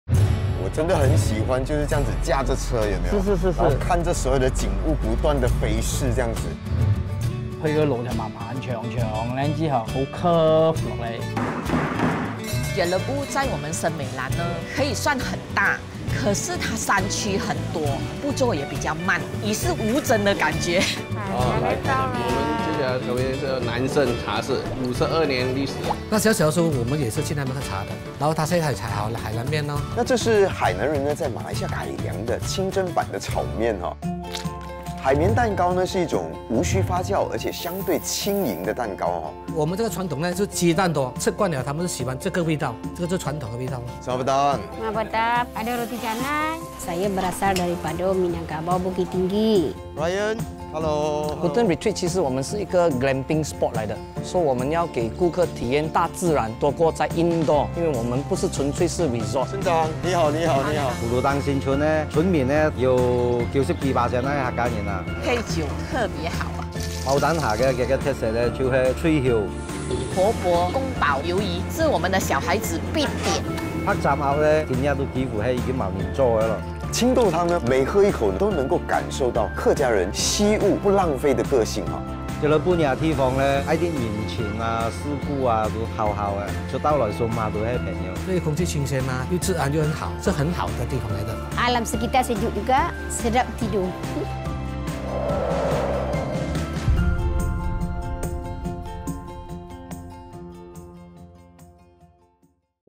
日叻务 Jelebu | 阿賢鄉鎮食旅 Axian Small Town Foodcation
旅程，未必要有固定景點，因為當下的路程已是最佳景緻。阿賢第一次駕車前往森美蘭州東北部的日叻務，長長的下坡道路風光獨好，兩旁的低矮山丘連綿不斷，綠樹植被，茂密山林，高低起伏，車窗外的景物每一分每一秒都在變化，隨風而去。

日叻務餐桌上的風景同樣不遑多讓，在沒有新式咖啡館的舊時代，海南人經營的食肆彷如鎮上的全民食堂。早上醒來不到海南咖啡店走一趟，就說不上是日常了。海南人擅長烹調清真食物，以中式炒麵為基礎融入黑醬油，燜炒得濕漉漉的，就變身為清真版的海南麵。從西方海綿蛋糕改良而成的本土生日蛋糕，再次見證海南人的底氣，善於變通，不死守常規。每一年的母難日，正是海南人製作的西式鮮奶油蛋糕，陪伴我們吹熄一年一次的歲月燭光。森美蘭州是米南加保族在馬來西亞的大本營，他們經營的家庭式小店風景如畫，Roti Canai 輕薄不膩口，咖哩中的椰漿與辣椒，雙倍濃郁，別具米南特色。Glamping 營地特別適合熱愛大自然露營，又不想失去任何生活設施與便利的人。營地附近的葫蘆頂新村，沒種葫蘆瓜，卻有最鄉土樸實的客家菜如白切番鴨和青豆湯，還有村民最愛的下酒菜毛丹蝦和佛缽蘇東。山林環抱的日叻務，地貌豐富，環境清幽，是親近大自然的好地方。